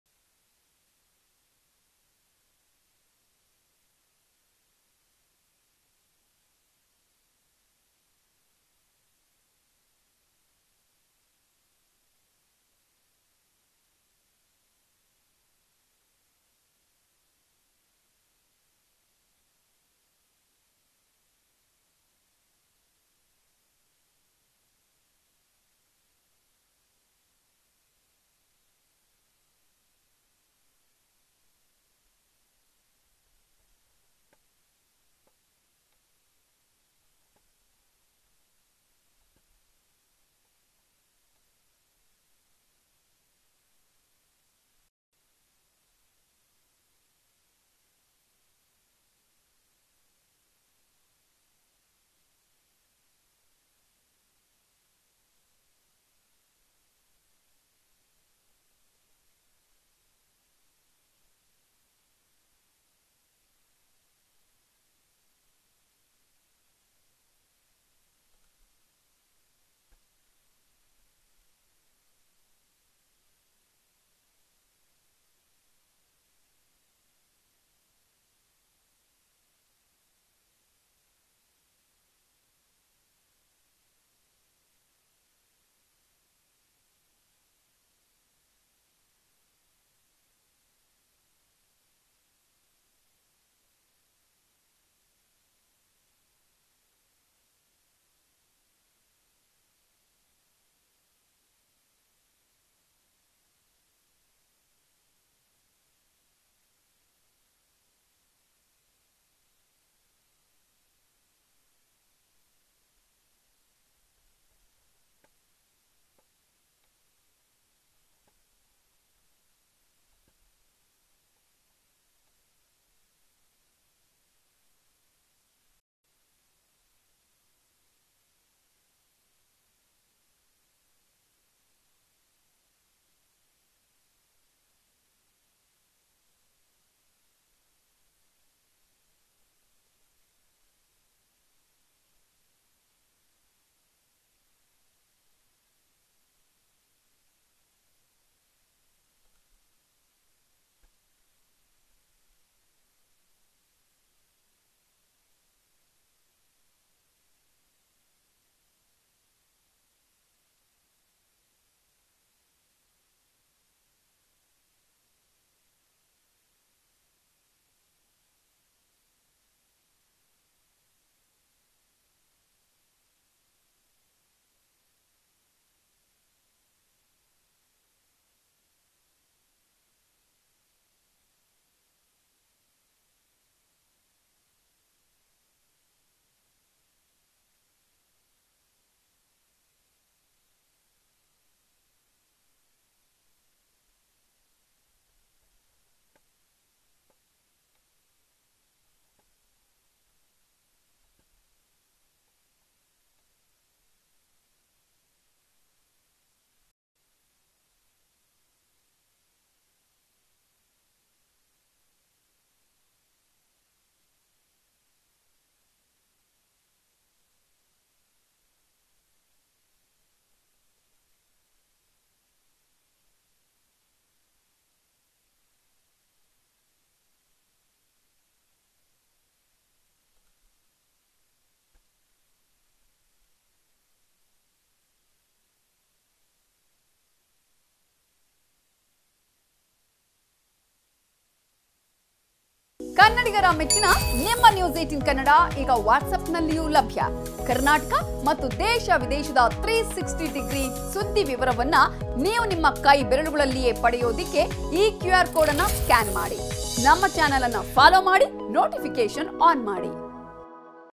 Vijayalakshmi Darshan Meets DK Shivakumar | ಡಿಕೆಶಿ ಭೇಟಿ ಬಳಿಕ ಹೋದ ವಿಜಯಲಕ್ಷ್ಮಿ, ದಿನಕರ್
Vijayalakshmi Darshan Meets DK Shivakumar | ಡಿಕೆಶಿ ಭೇಟಿ ಬಳಿಕ ಹೋದ ವಿಜಯಲಕ್ಷ್ಮಿ, ದಿನಕರ್ | N18V

News18 Kannada Live | ನ್ಯೂಸ್ 18 ಕನ್ನಡ ಲೈವ್
Karnataka | ಕರ್ನಾಟಕ
Kannada | ಕನ್ನಡ